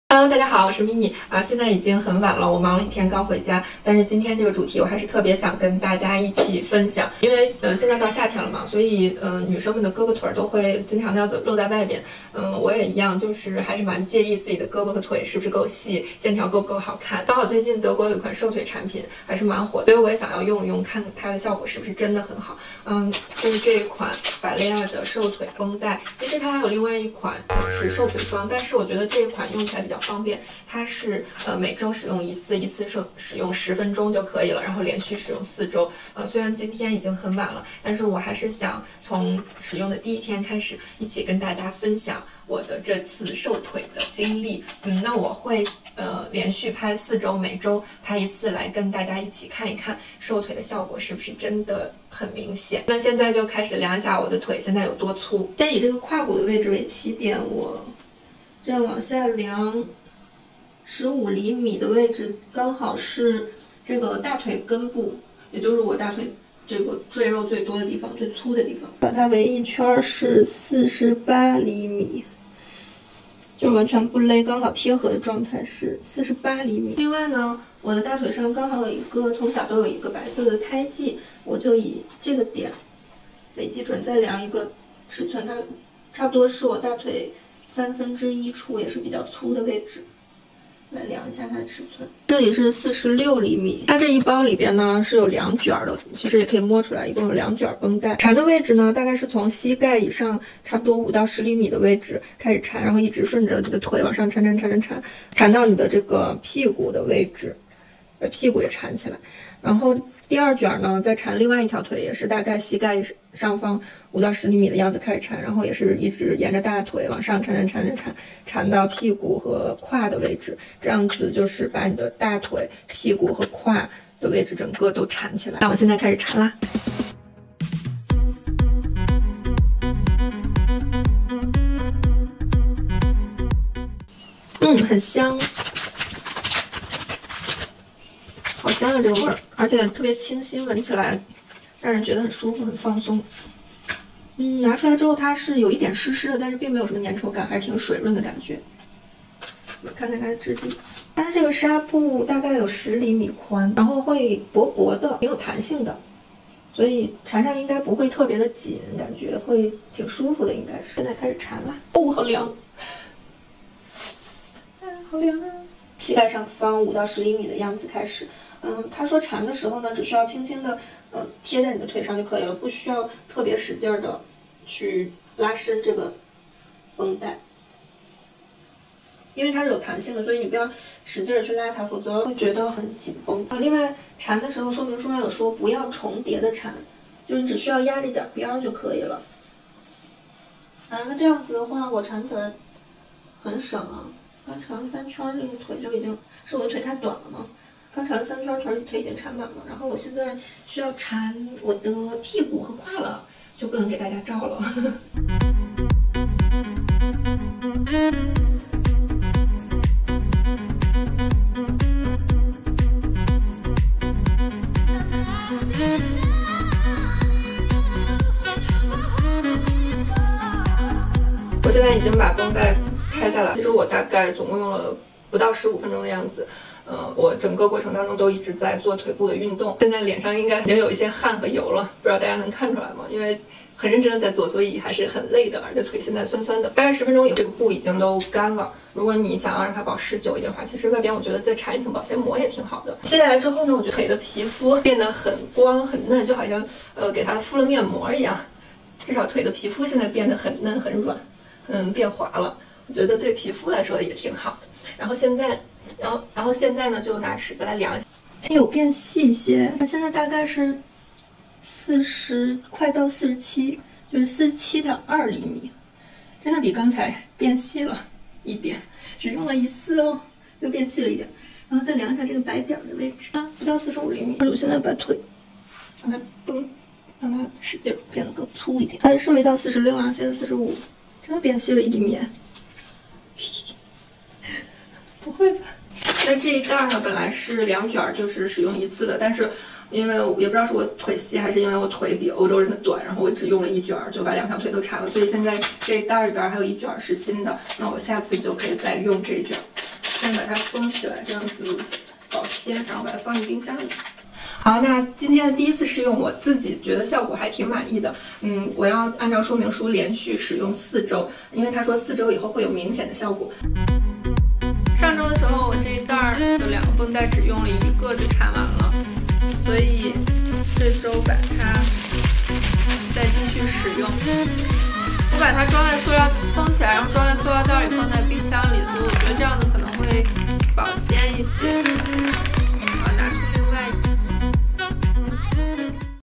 四周瘦大腿！瘦腿绷带瘦大腿实战效果测评 | 第一周
I Found You!瘦腿绷带瘦大腿实战效果测评 | 第一周
我从上个月开始试用这个瘦腿绷带，每周用一次，每次十分钟。一直到上周终于用完了四次！因此迫不及待的把视频剪辑出来发给大家看效果！

如果大家喜欢我的视频一定要记得帮我点个赞👍哦！谢谢大家的支持，和你们的互动是支撑我一直拍VLog的动力